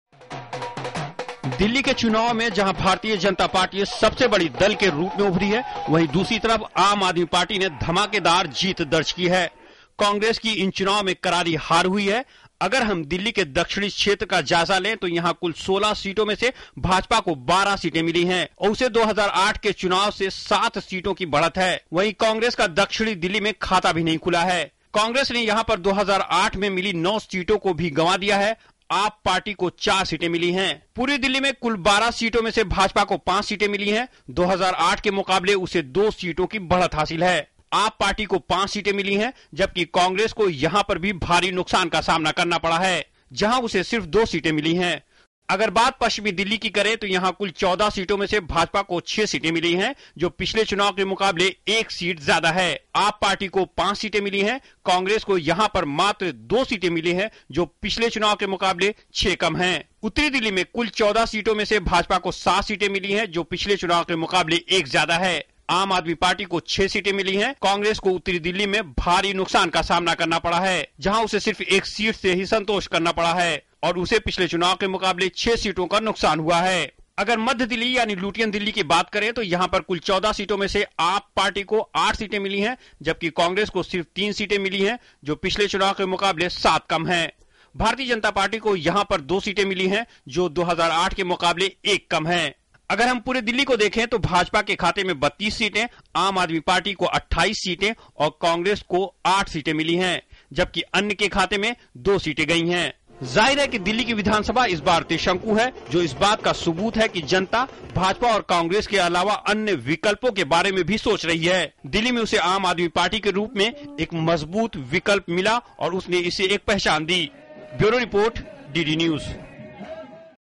Delhi Assembly polls: Kejriwal pulls off stunning victory against Dikshit
Debutant Aam Aadmi Party convener Arvind Kejriwal on Sunday pulled off a major upset against three-time Delhi Chief Minister Sheila Dikshit, defeating her with a thumping margin of over 25,000 votes in New Delhi constituency.

The AAP leader, whom both Congress and BJP leaders had dismissed as a "non-actor", defeated Dikshit by a margin of 25,864 votes.